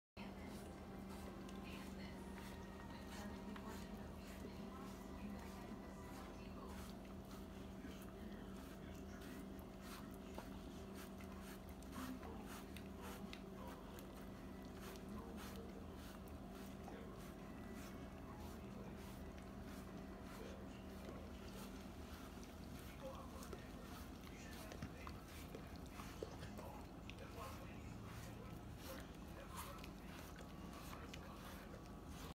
Dog trying to eat the pizza on a pizza box
Our dog, Maggie, trying to eat the pizza on an empty pizza box.  Poor thing!